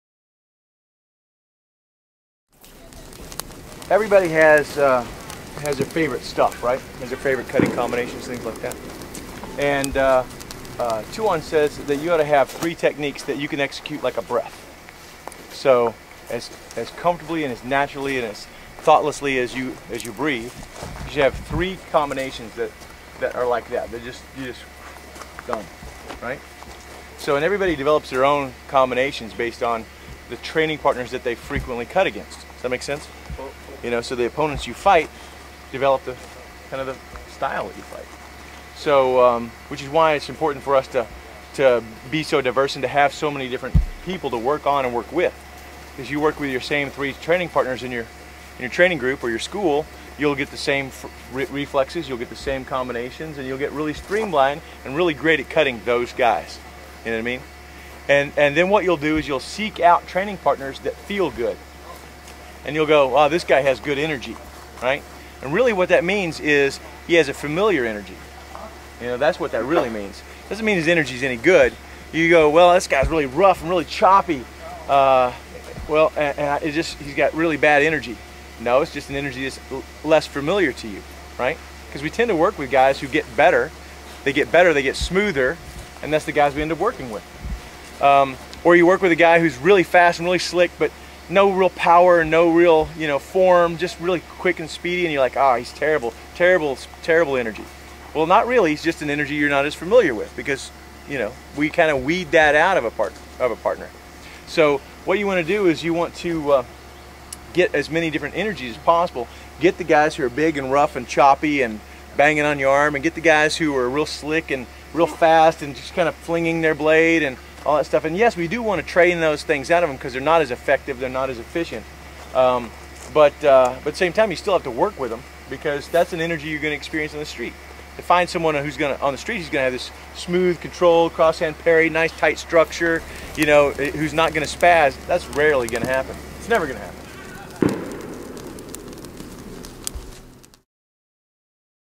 Sama Sama 2010 Trailer 03
Guro Harley Elmore explaining training partner energy and getting out of your comfort zone. This material is on DVD 2 of the Sayoc Sama Sama 3 DVD set. Available on sayoc.com